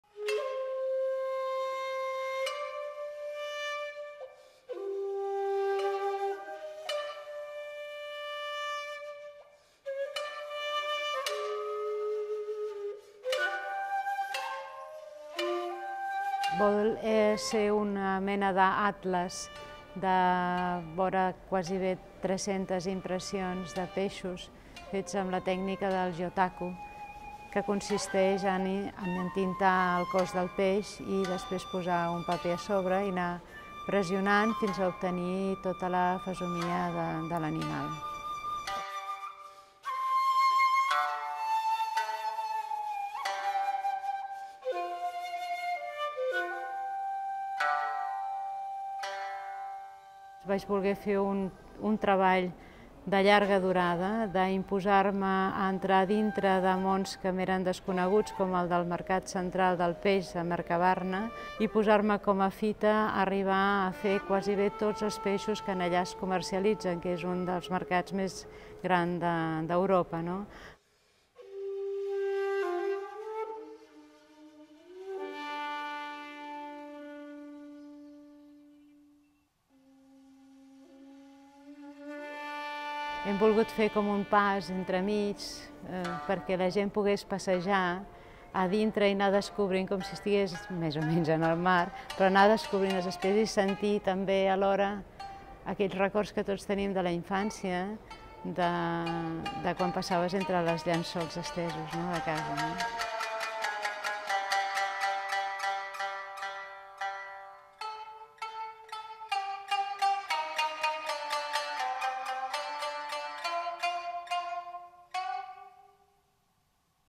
'Gyotaku, capturar l'ànima dels peixos', al Museu Marítim
El Museu Marítim acull fins al 28 de setembre l'exposició 'Gyotaku. Capturar l'ànima dels peixos'. La mostra de l'artista barcelonina Victòria Rabal recull 300 estampes de més de 70 espècies diferents entre peixos i mol·luscs fetes amb la tècnica d'impressió nipona Gyotaku. Més informació